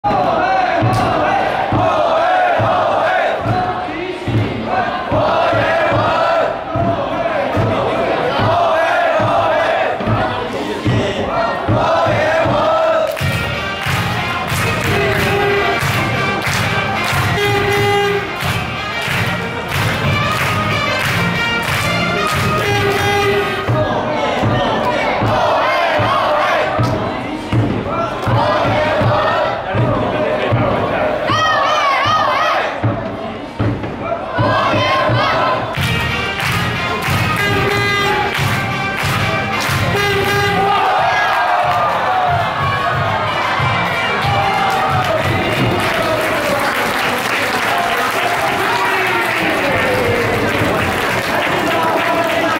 C0009 20190620 , 一下 , 郭嚴文：中外野方向飛球被接殺，三出局。中信兄弟（0）：（2）Lamigo桃猿
20190620-中信兄弟 vs Lamigo 桃猿